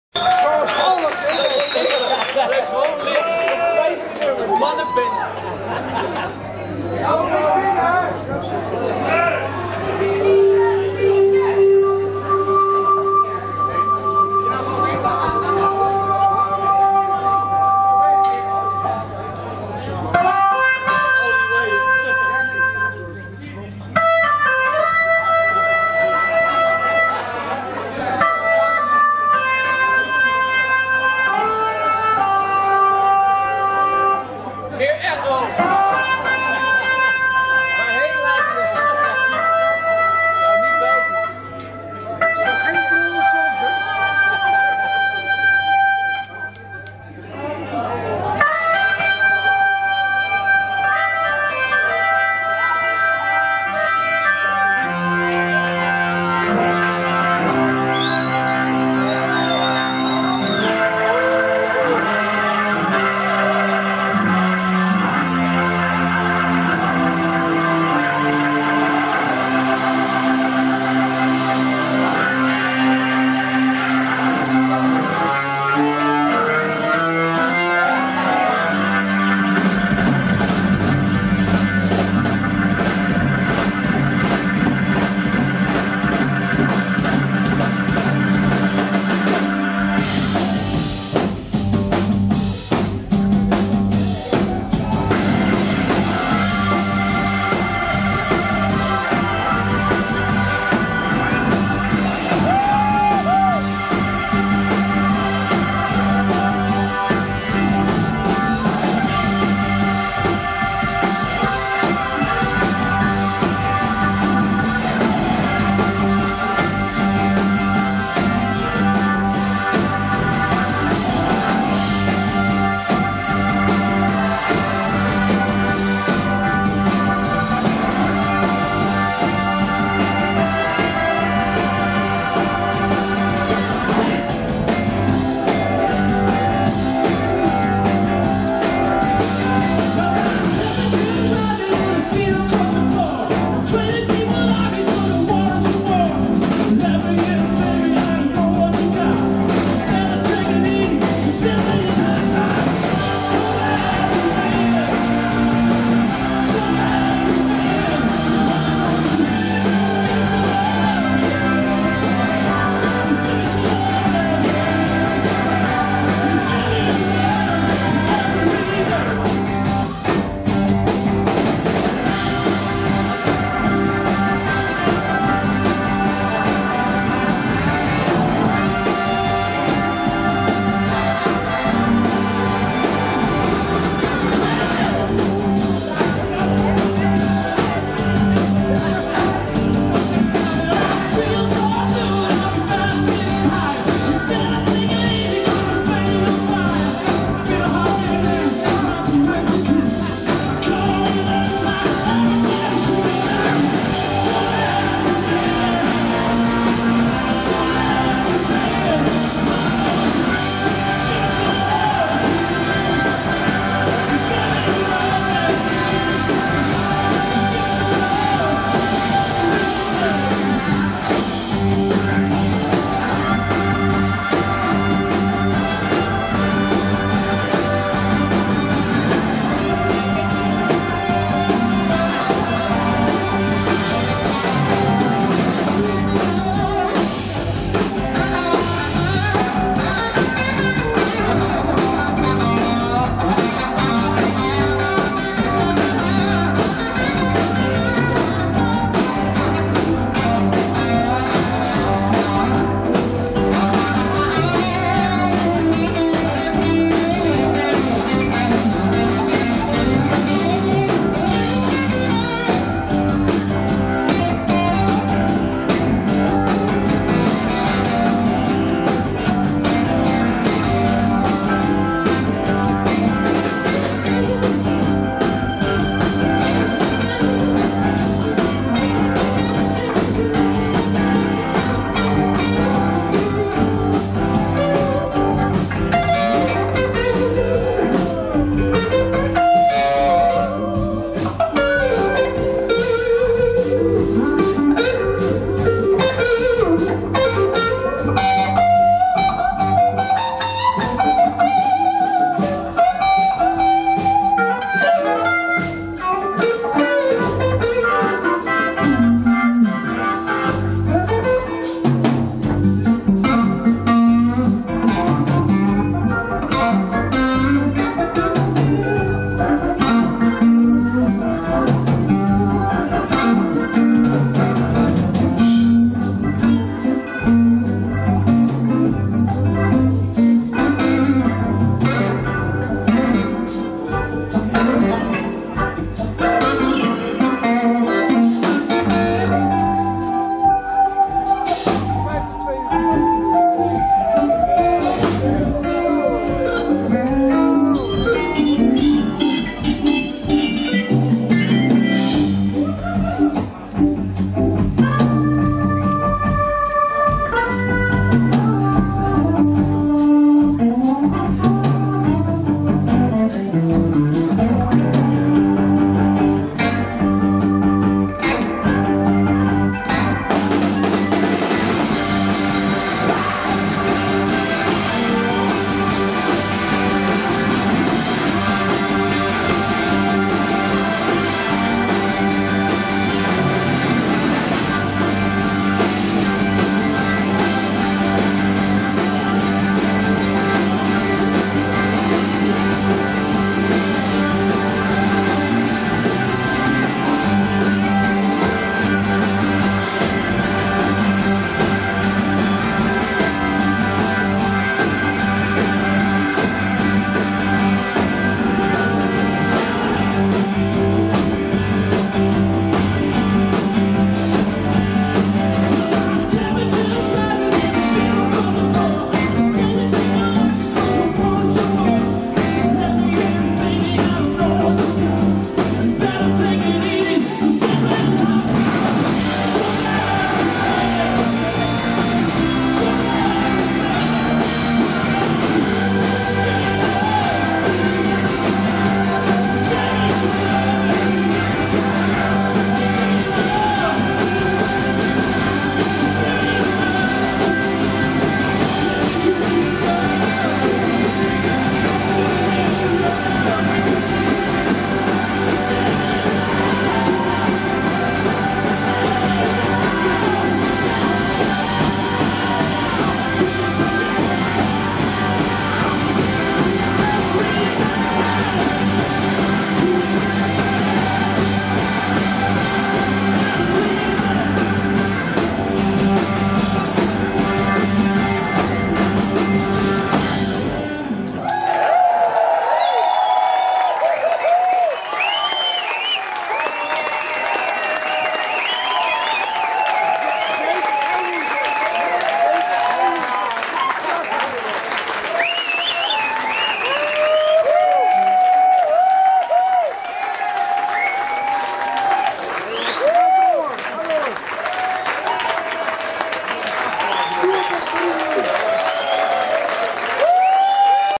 Orgel Vreten, Patronaat, Haarlem, 13-03-2014
Orgel Vreten

Patronaat, Haarlem, The Netherlands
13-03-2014

Does anybody knows the setlist of the concert. Of te wel de menukaart ;)

Orgel Vreten is:
Robin Piso (Hammond)
Thijs Schrijnemakers (Hammond)
Jan Teertstra (Bass)
Wouter Rentema (Drums)

Guests: Ruud Borgers (Guitar), Benjamin Herman (Saxophone), Daniël van Loenen (Trombone), Spike van Zoest (Guitar)

It is prohibed to sell this film

Support the artists by buying their official releases

PAL